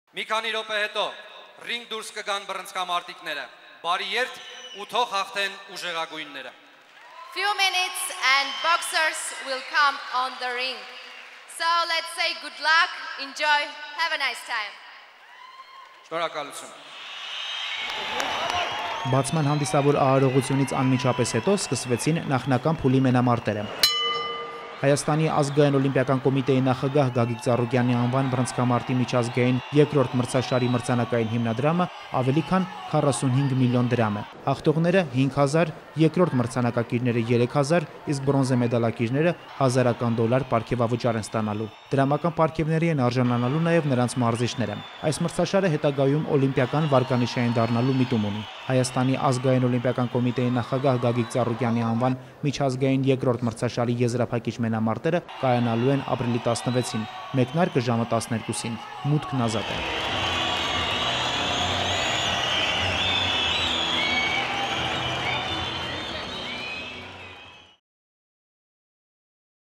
INTERNATIONAL BOXING COMPETITION AFTER GAGIK TSARUKYAN TOOK PLACE
Ապրիլի 12-ին Կարեն Դեմիրճյանի անվան մարզահամերգային համալիրում մեկնարկեց ՀԱՕԿ նախագահ Գագիկ Ծառուկյանի անվան բռնցքամարտի միջազգային մրցաշարը: Մրցաշարն անցկացվում է արդեն երկրորդ անգամ, և նախորդի հետ համեմատ մասնակից երկրների և բռնցքամարտիկների թիվը կրկնապատկվել է: Եվրոպայի, Ասիայի, Աֆրիկայի և Ամերիկայի 18 երկրների ավելի քան 130 ուժեղագույն բռցքամարտիկները կպայքարեն հաղթողի կոչման համար: Մենամարտերը տեղի կունենան ապրիլի 12-ից-16-ը: Բռնցքամարտի միջազգային ֆեդերացիայի կանոնակարգի համաձայն պայքարը կընթանա 10-ը քաշային կարգերում: